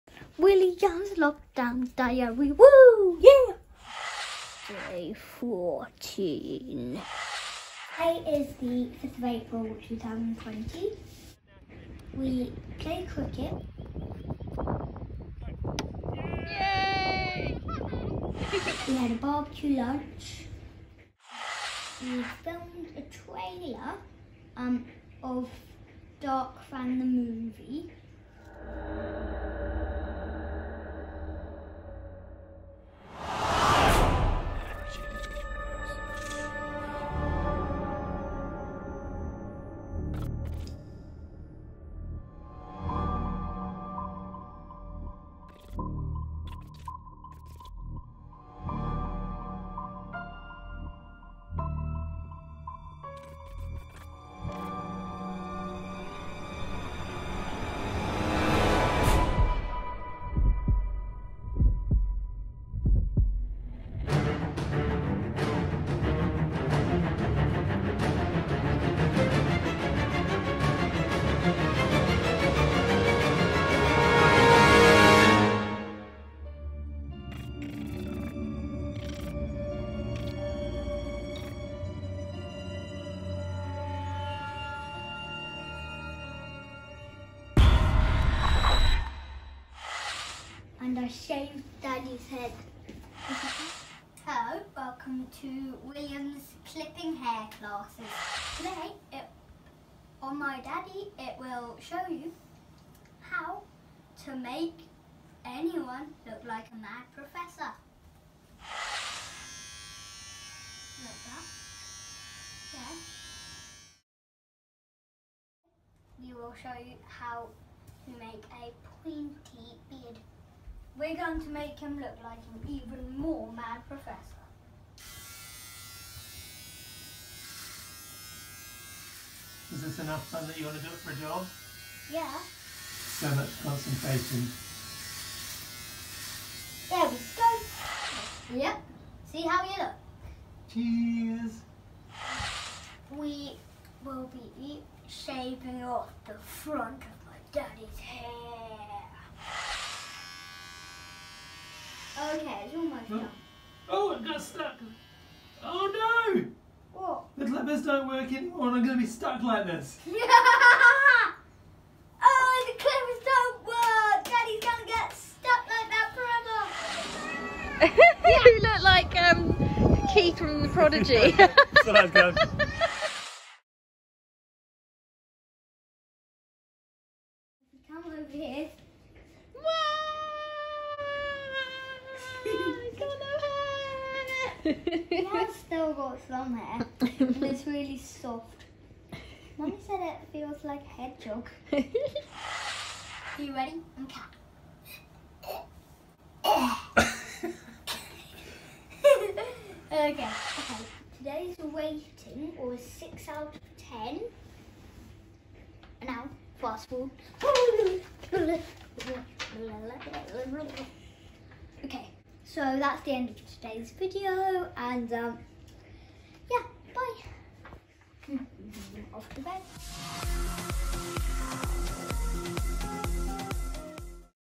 Day 14 - 5th April 2020: William’s lockdown diary
Shaving daddy’s head into a mad professor, made a horror movie trailer, hocked up a fur ball, played cricket